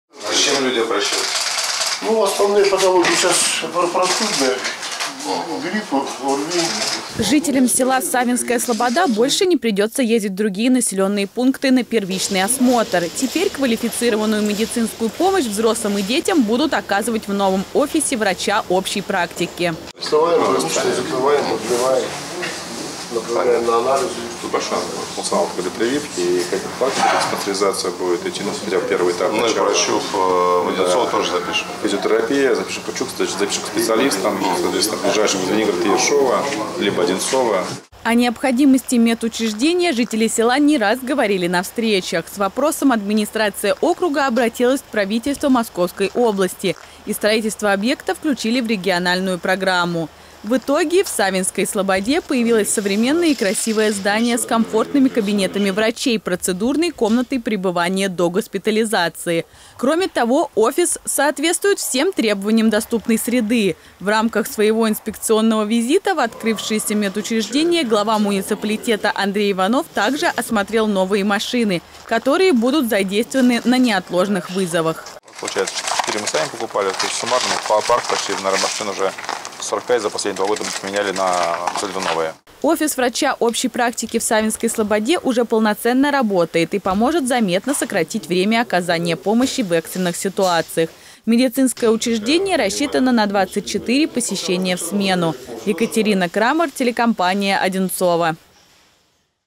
Инспекция офиса врача общей практики в Саввинской Слободе
Современный и комфортный для жителей. Глава муниципалитета Андрей Иванов осмотрел офис врача общей практики в селе Саввинская Слобода. В новом медицинском учреждении можно будет пройти первичный осмотр и сдать анализы.